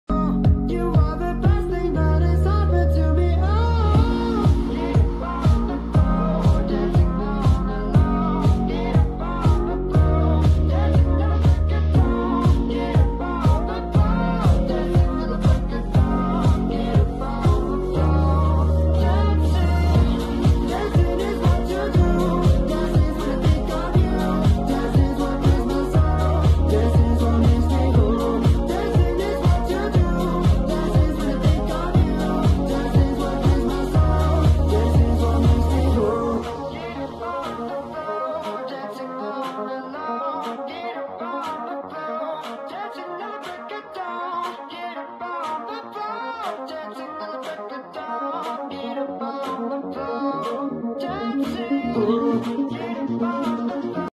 I made Tug of war Squid game Made out of btools
I had help Just with 1 friend